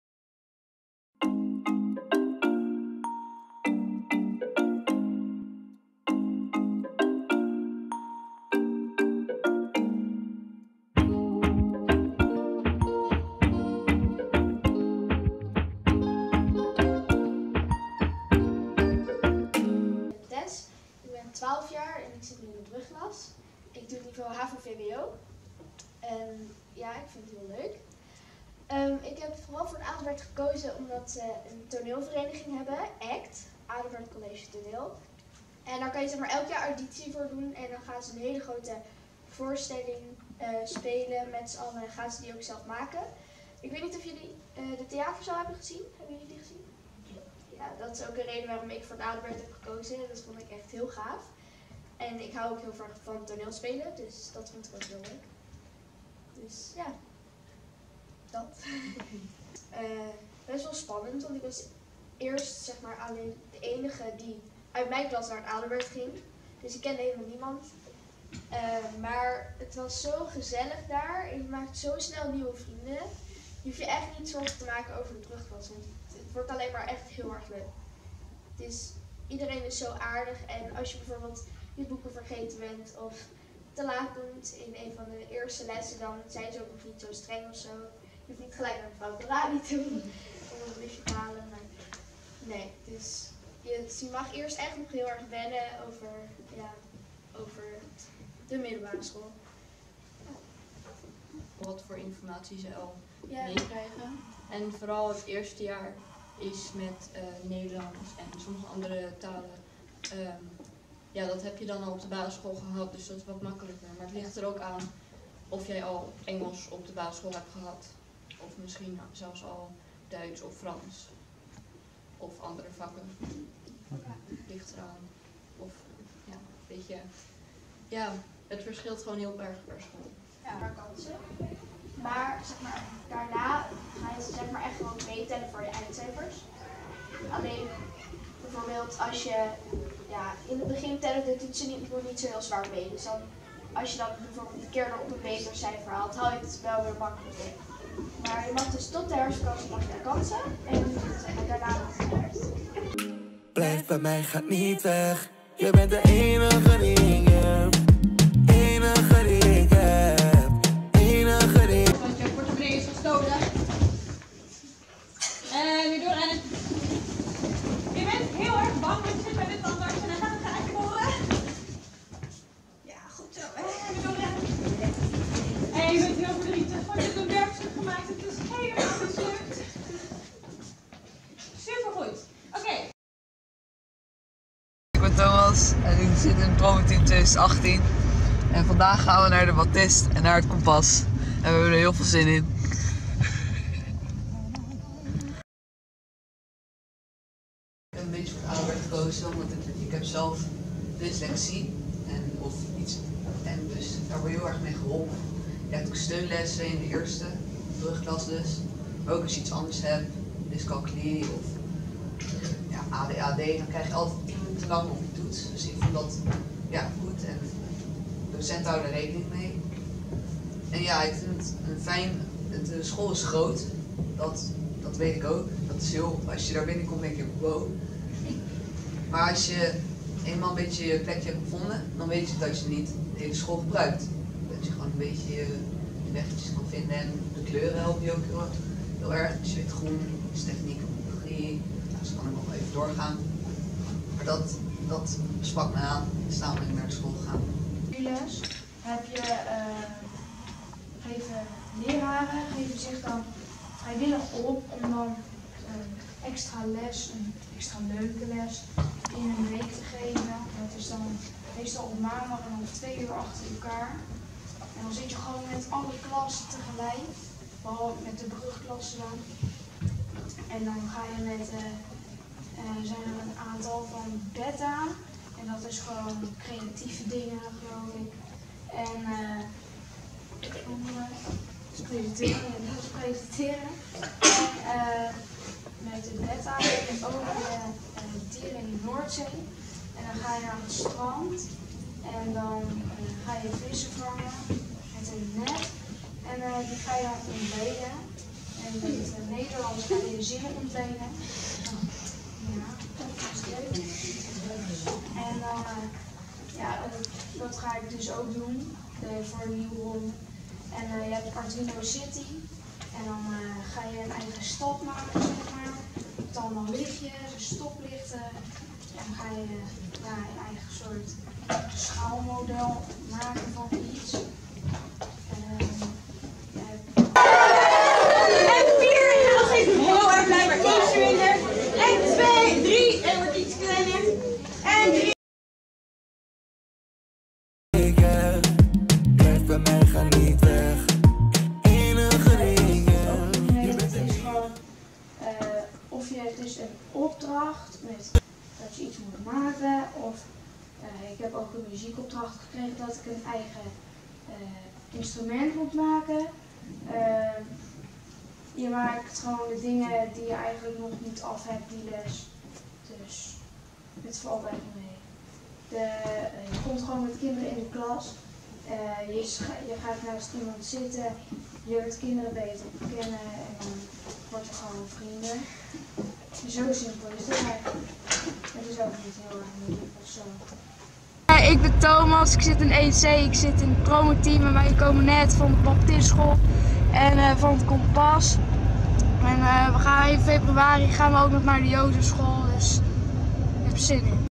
week2promoteam
Promo-team Adelbert College on tour! We komen langs verschillende basisscholen om vragen te beantwoorden :-)